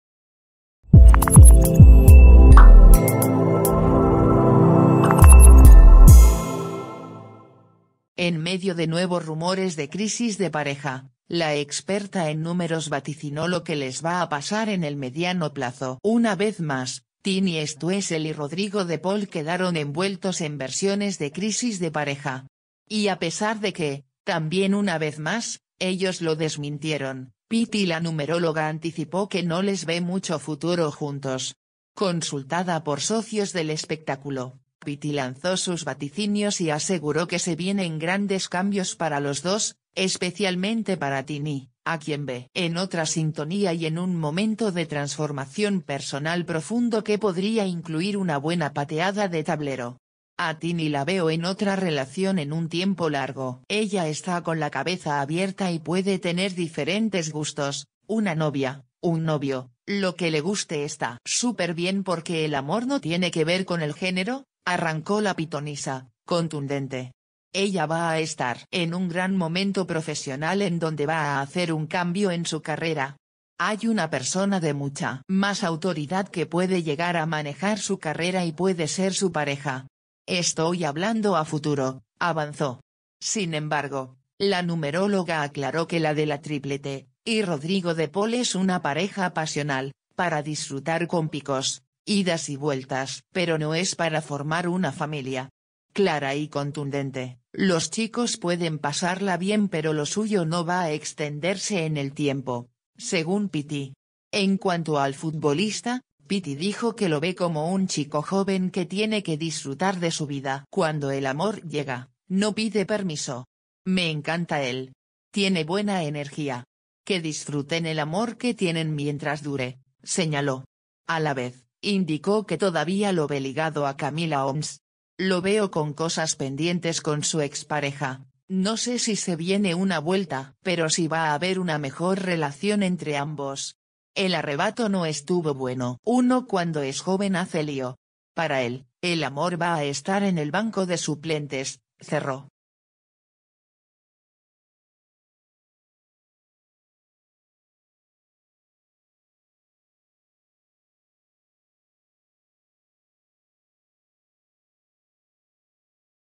Pitty La Numeróloga predijo el futuro de Tini Stoessel y De Paul y fue letal: “A ella la veo en…”
Pitty La Numeróloga predijo el futuro de Tini Stoessel y De Paul y fue letal: “A ella la veo en…” - Últimas Noticias teleshow

En medio de nuevos rumores de crisis de pareja, la experta en números vaticinó lo que les va a pasar en el mediano plazo. Una vez más, ...